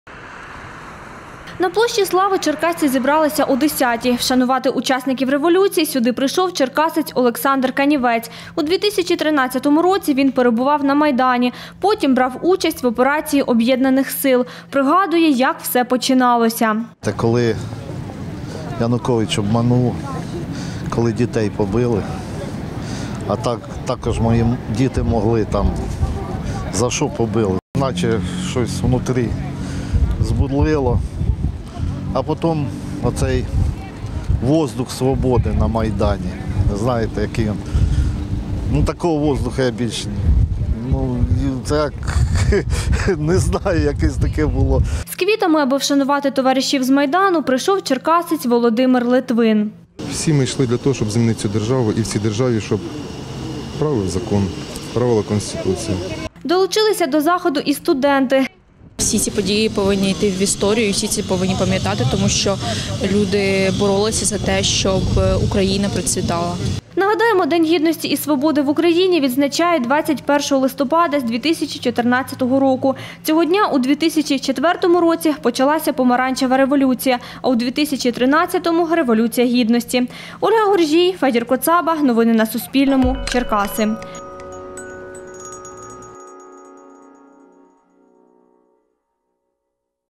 Черкасці вшанували учасників двох революцій 2004 та 2013
Покладанням квітів до пам’ятника «Борцям за волю України», що в обласному центрі, черкасці вшанували учасників двох революцій – Помаранчевої 2004 року та Революції гідності 2013 року. Захід відбувся на площі Слави до Дня Гідності та свободи, який в Україні відзначають сьогодні.